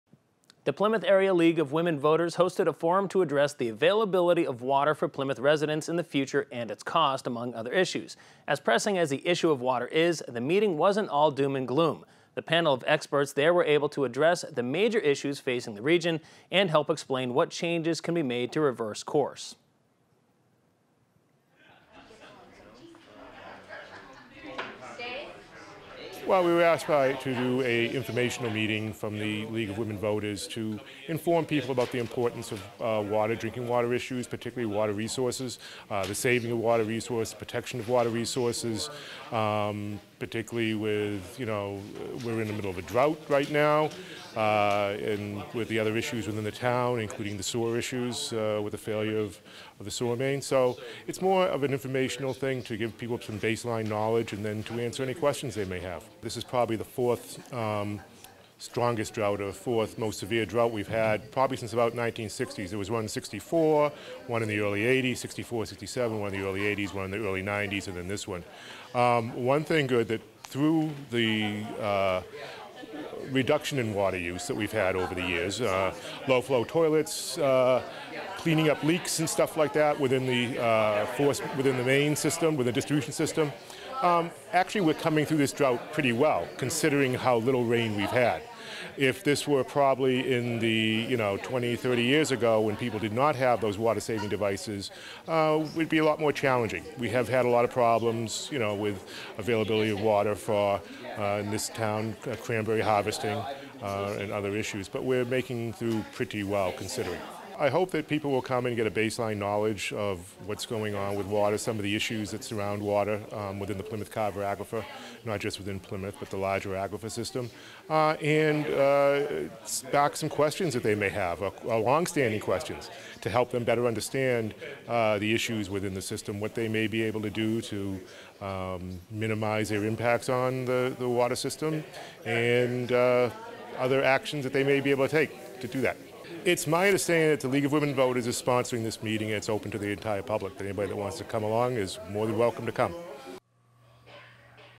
PCN Plymouth League of Women Voters Water Forum
PCN PACTV Community News Plymouth League of Women Voters Water Forum

The Plymouth Area League of Women Voters hosted a forum to address the availability of water for Plymouth residents in the future and its cost, among other issues.  As pressing as the issue of water is, the meeting wasn’t all doom and gloom.  The panel of experts there were able to address the major issues facing the region, and help explain what changes can be made to reverse course.